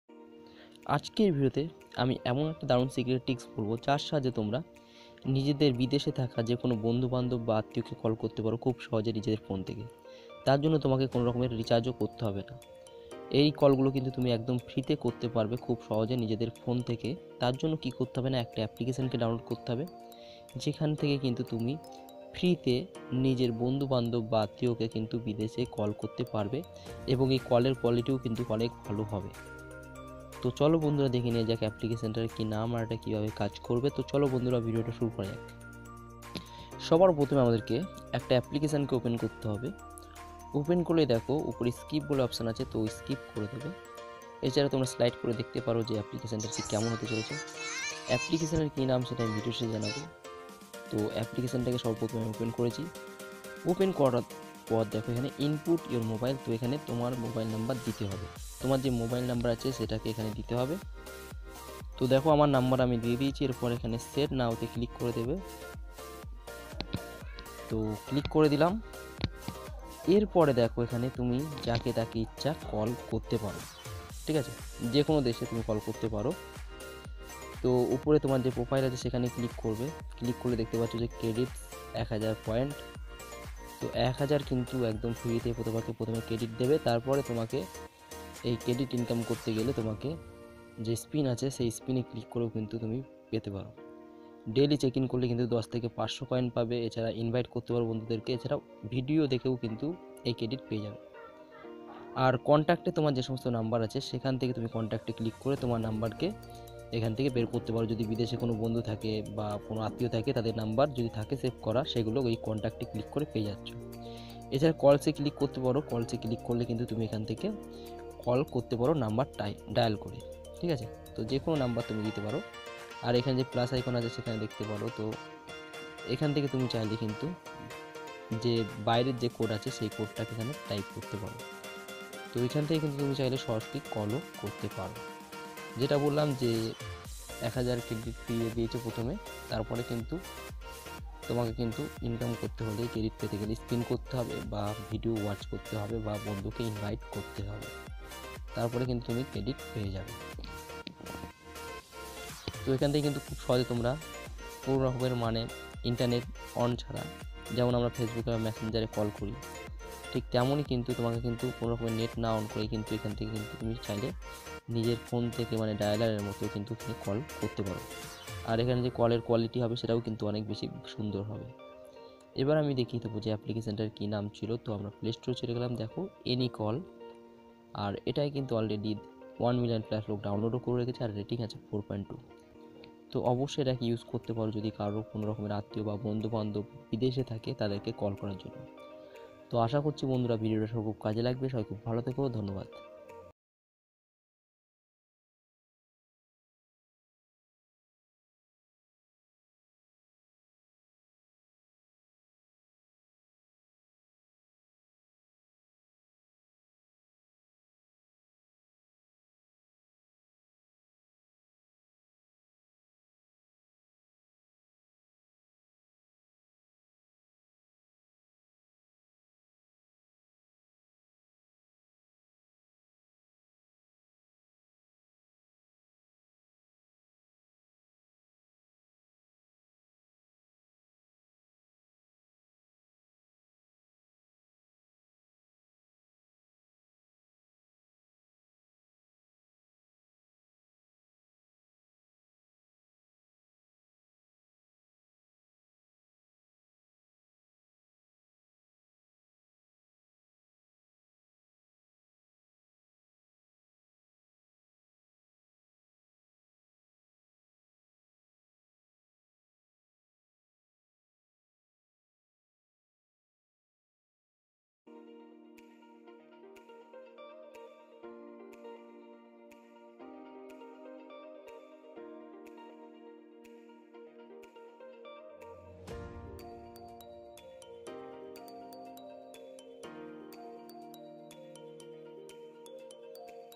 How to Use Any Call App / Best Calling App For International Call 2022
hi friends
             ajker video te Ami sekhabo kivabe any call app use korbe.